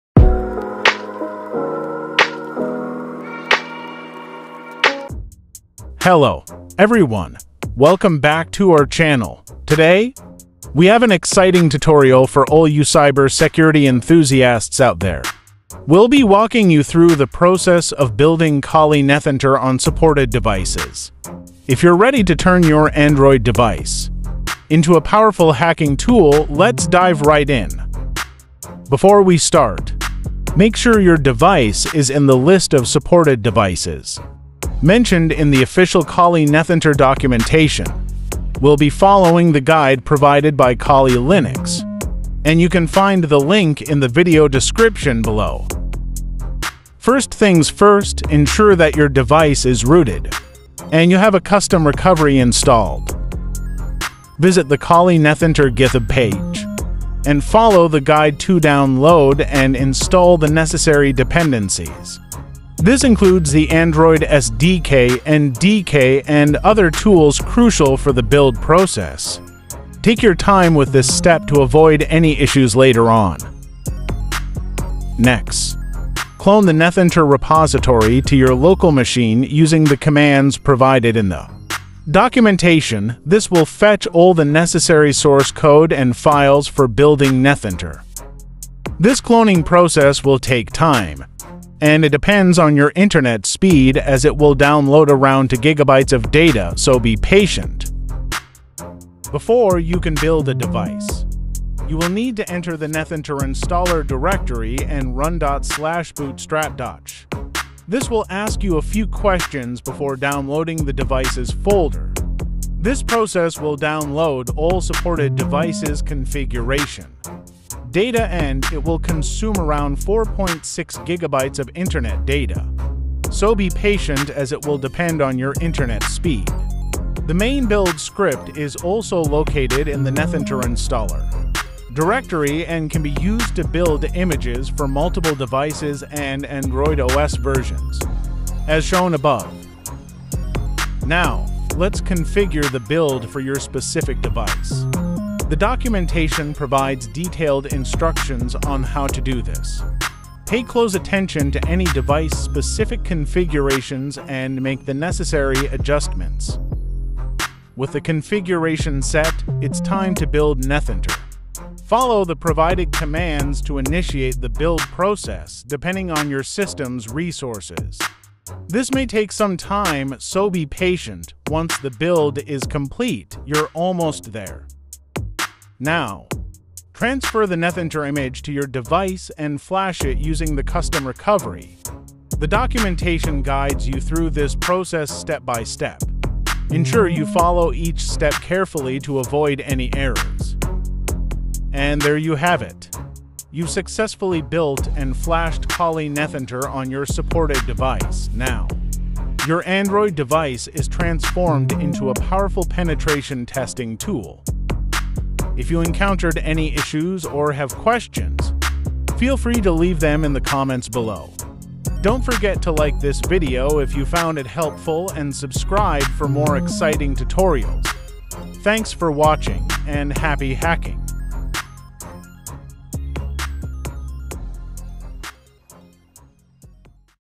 Build Kali NetHunter For Supported Devices: Ft. OnePlus 6/6T
Make sure your device is in the list of supported devices mentioned in the official Kali NetHunter documentation. We'll be following the guide provided by Kali Linux.

How To Install Kali Nethunter On Any Android Device :-

How To Install Kali Nethunter On Any Rooted Android Device :-

How To Install Kali Nethunter On OnePlus 6/6T :-